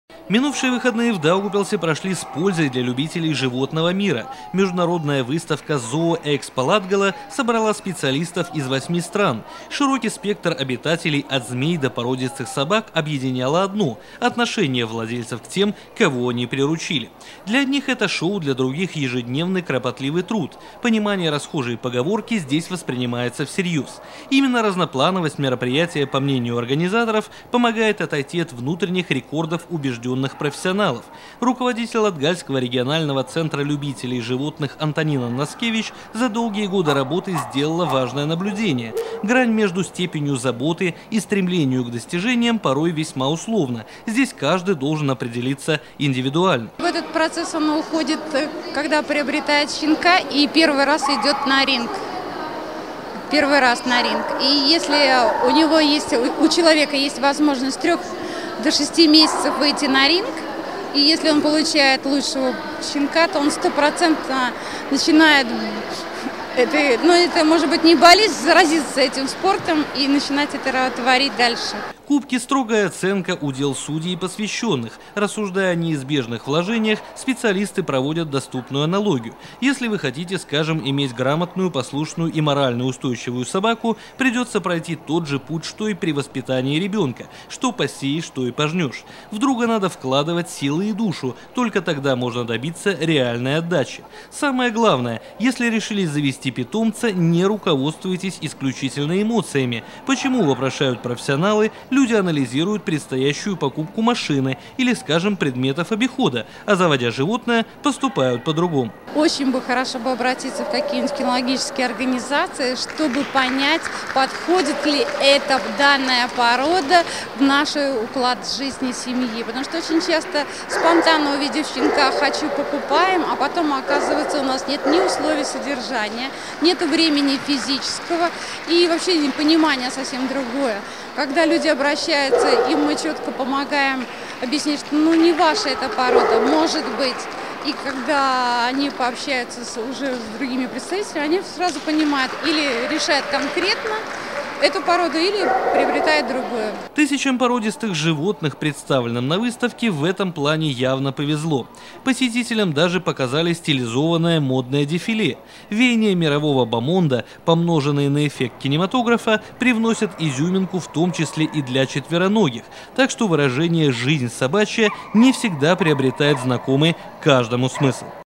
Выставка животных ZOOEKSPO "Latgale 2013"
С 19 по 20 января в Даугавпилсе прошла выставка животных ZOOEKSPO "Latgale 2013". Посетители выставки могли полюбоваться кошками, голубями, кроликами, однако главными героями мероприятия стали собаки. За два дня зрителям было представлено около 1000 собак 135 пород из 8 стран -- Латвии, Литвы, Эстонии, Белорусси, Украины, России, Польши и Финляндии.